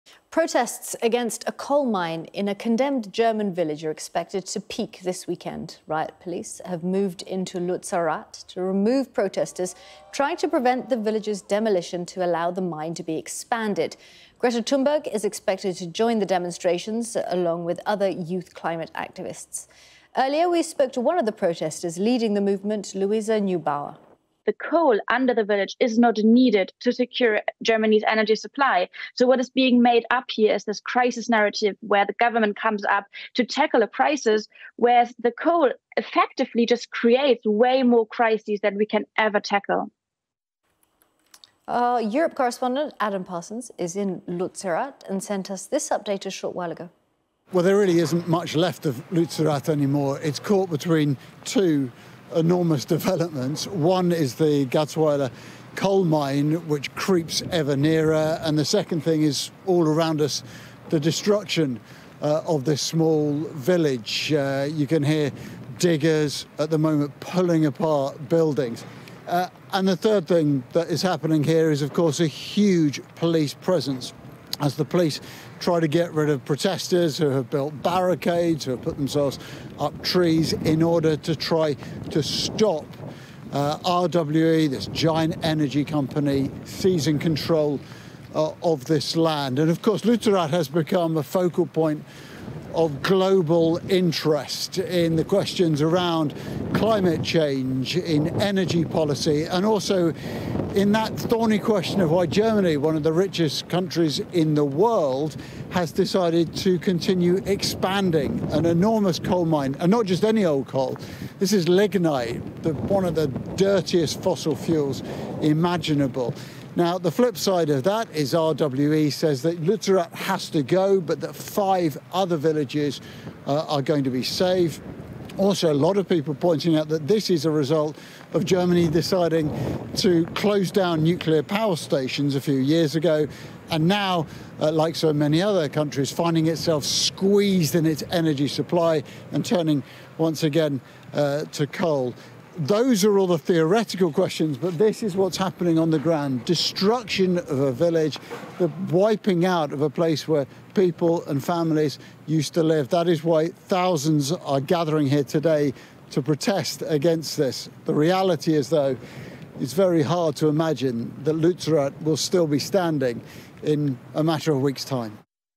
Thunberg joins protests at village under threat of destruction over coal mine
Greta Thunberg has condemned the forced removal of protesters from a small village in western Germany that is due to be demolished to accommodate the expansion of an open coal mine.